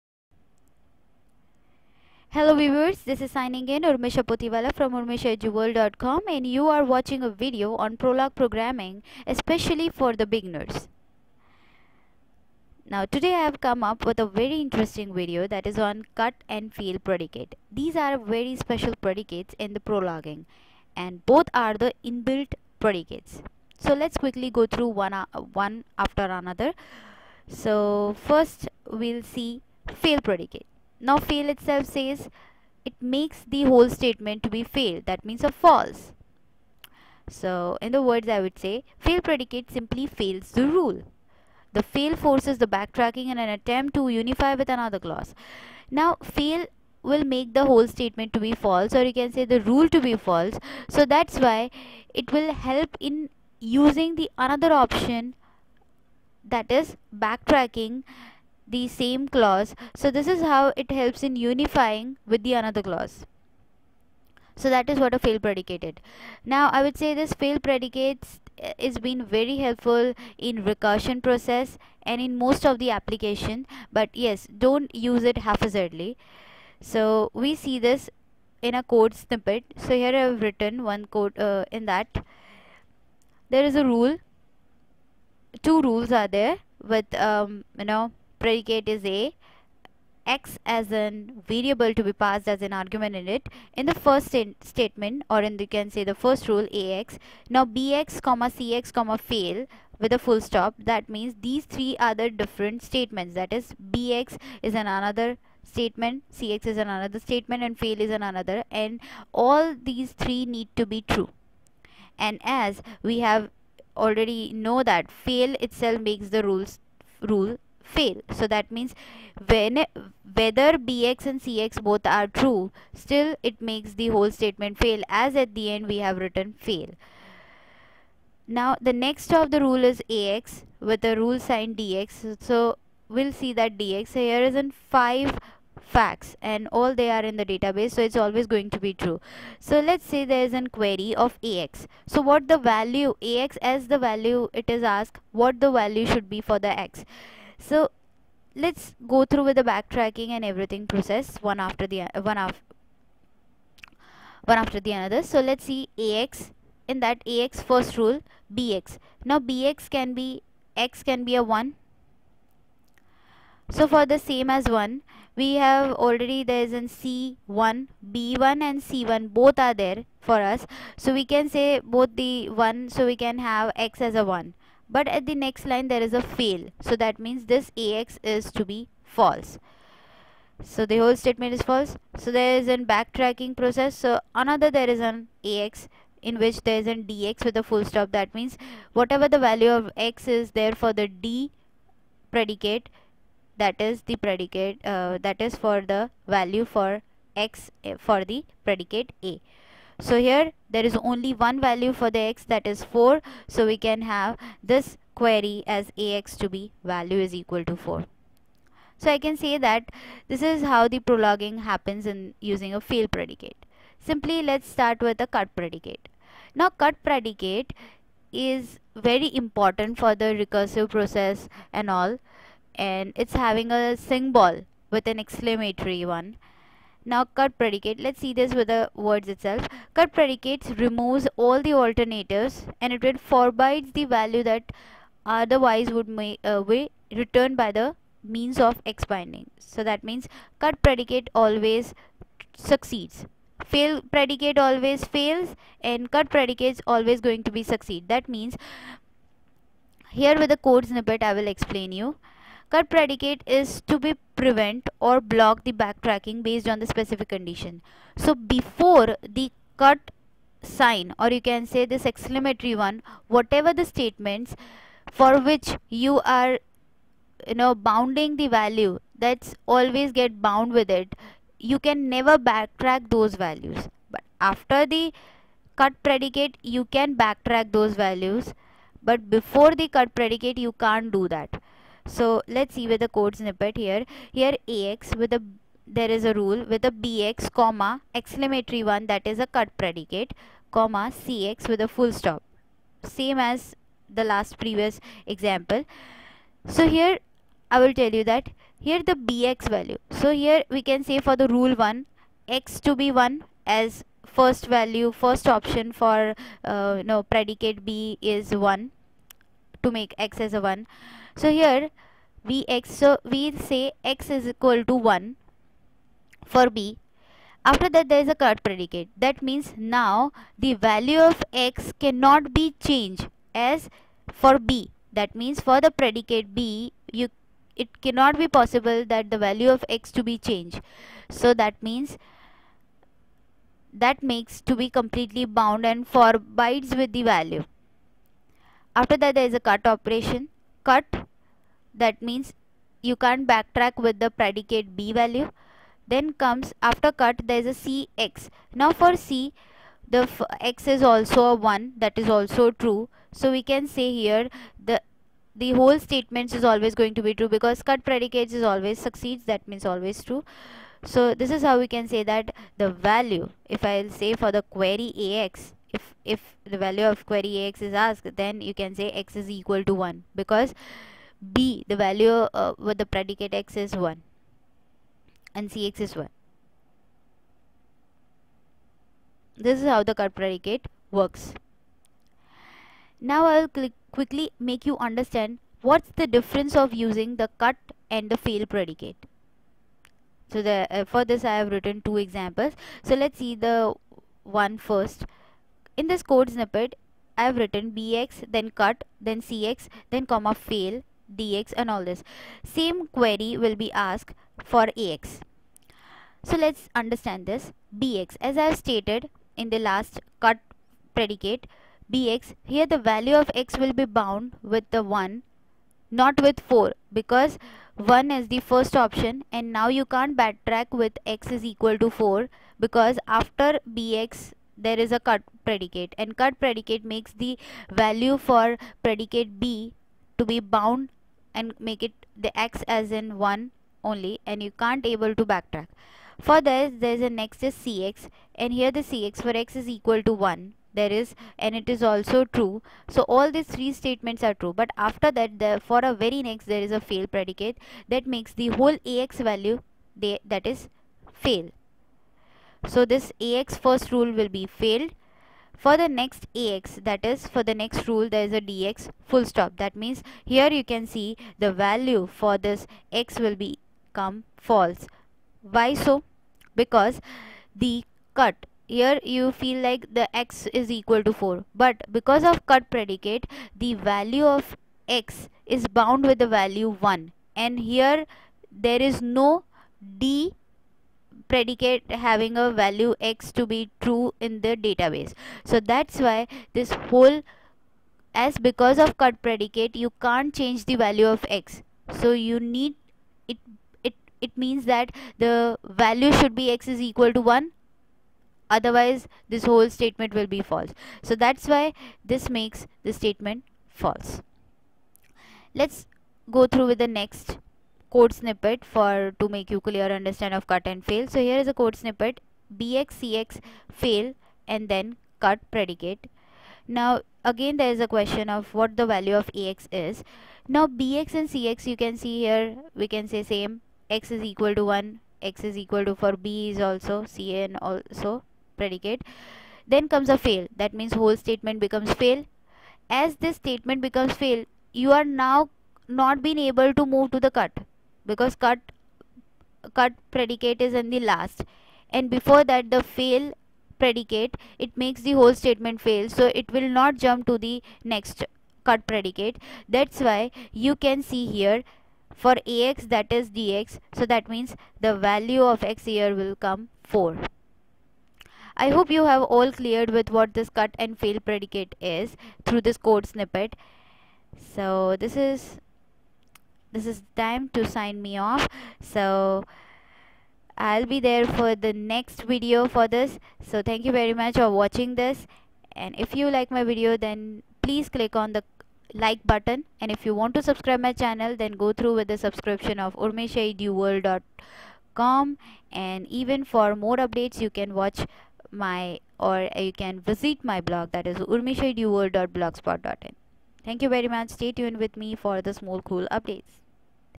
Prolog - Part 7 What is Cut and Fail Predicate? Their difference
The contents covered in this video  are:
1. Fail Predicate
2. Cut Predicate
3. The difference between Cut and Fail Predicate